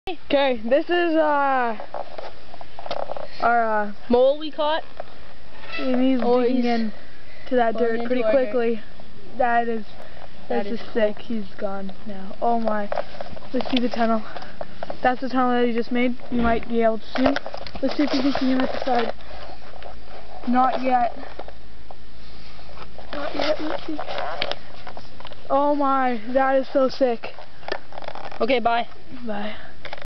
a mole I caught digging a hole
this is the mole i caught and hes digging a hole